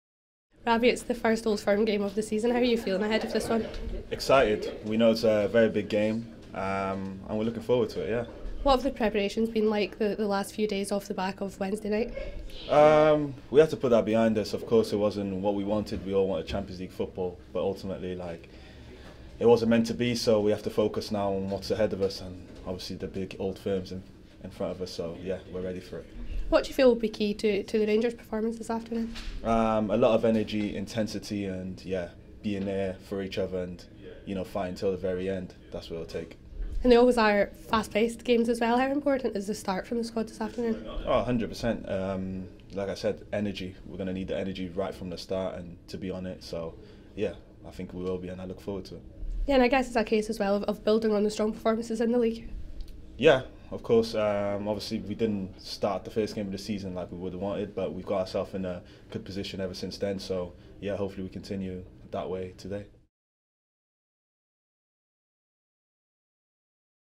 PREMATCH | Rabbi Matondo | Rangers v Celtic 03 Sept 23
Rabbi Matondo speaks to RTV ahead of the first Old Firm of the season.